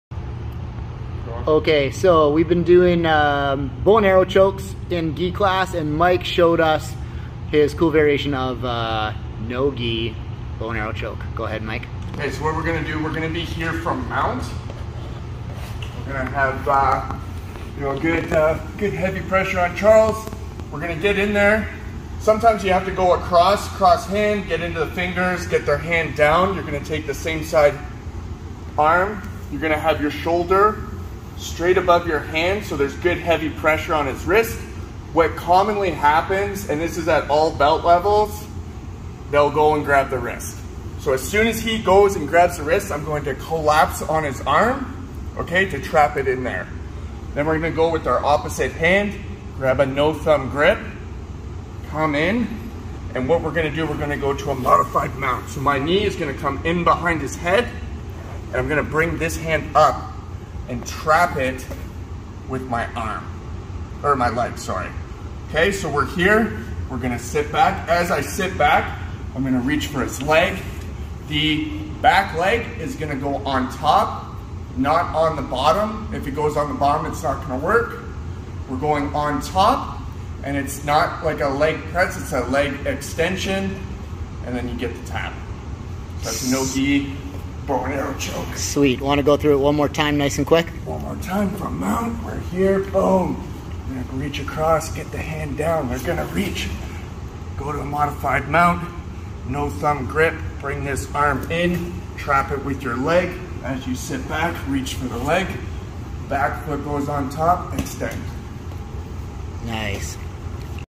No-Gi Jiu Jitsu Bow and Arrow Choke - Steel City MMA, Sault Ste. Marie, Canada
Bow and arrow choke in no gi jiu jitsu? yes you can!
Let coach Mike from Steel Ciry MMA in Sault Ste. Marie show you how.